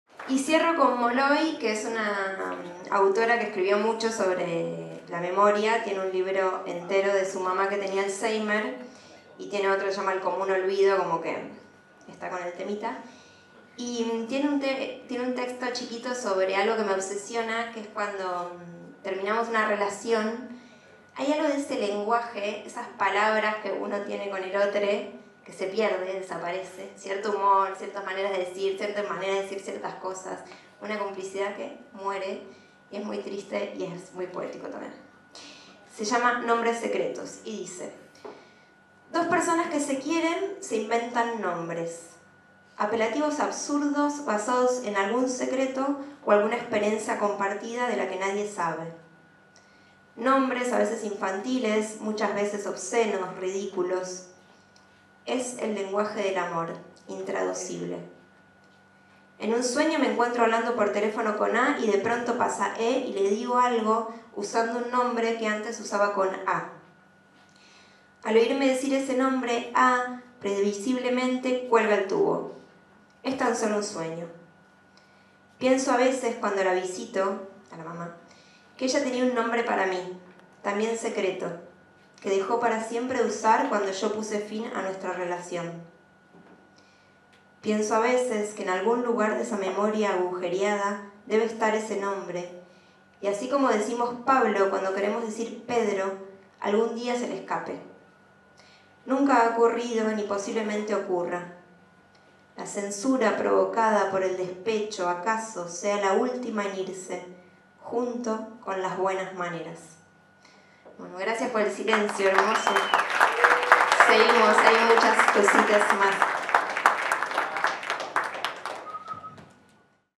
Maga Cervellera lee "Nombres secretos" de Sylvia Molloy
🎤 Maga Cervellera
📖 Sylvia Molloy "Nombres secretos"
🎭 Festival memoria viva  IIIbis
📆 24 de agosto de  2024
🎪 en JJ circuito cultural.

Brave Rewards

🎬Imago Euterpe🖖